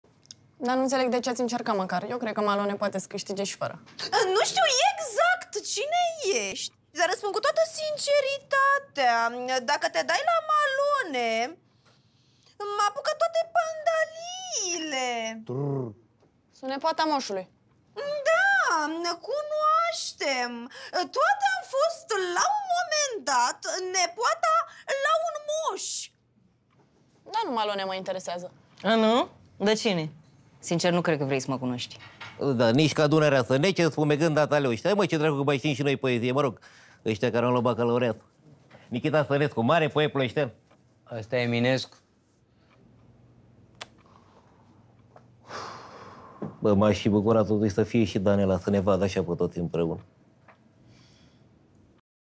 Gina o atenționează pe Roberta să nu se dea la Malone!
Urmăreşte AntenaPlay pe